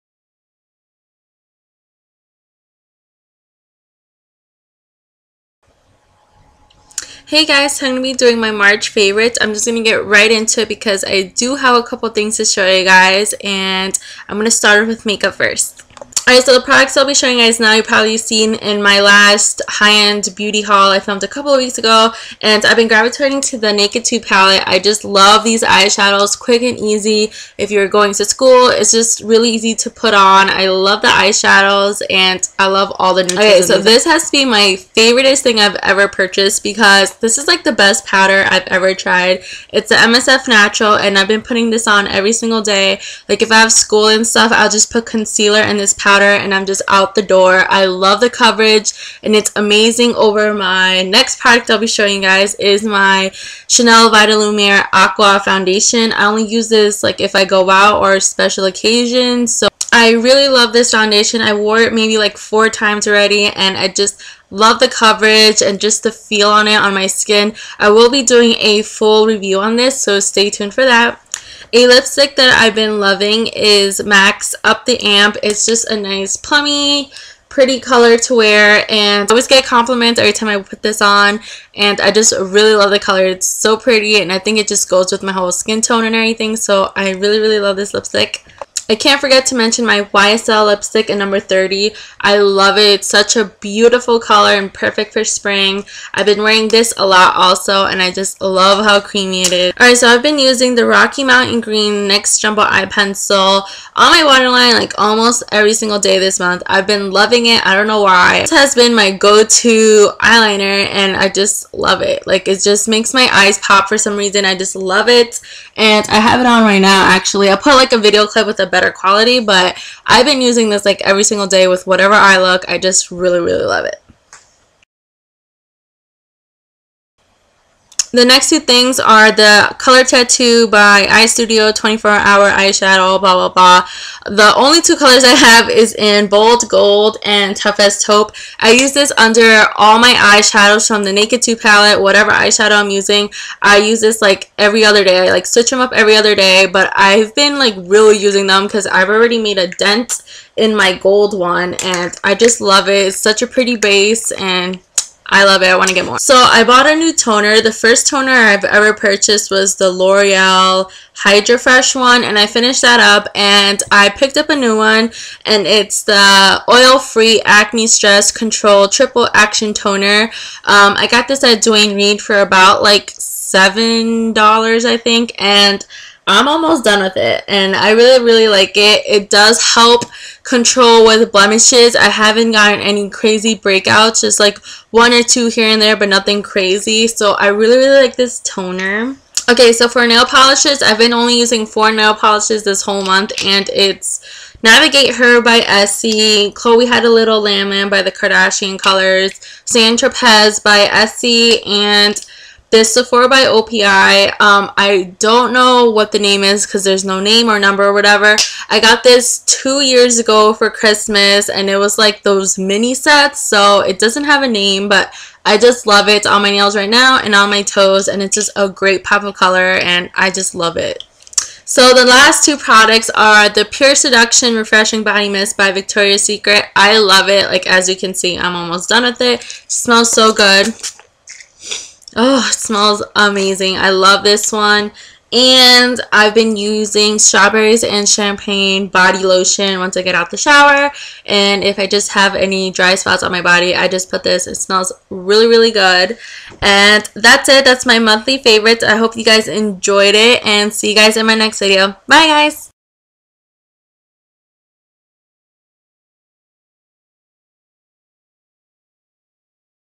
March Favorites 2012
-UD Naked 2 Palette
-MAC MSF Natural (Medium)
-Chanel Vitalumiere Aqua Foundation (B20)
-MAC Lipstiuck (Up The Amp)
-YSL Lipstick (#30)
-NYX Jumbo Pencil (Rocky Mountain Green)
-Maybelline Color Tattoo Eyeshadow (Tough As Taupe & Bold Gold)
-Neutragena Acne Stress Control Triple Action Toner
-Essie Nail Polish (Navigate Her & Sand Tropez0
-Nicole By OPI (Khloe Had A Little Lam Lam
-Sephora By OPI (Orange color lol no name)
-Victoria Secret Body Mist (Pure Seduction)
-Strawberries & Champagne Hand & Body Cream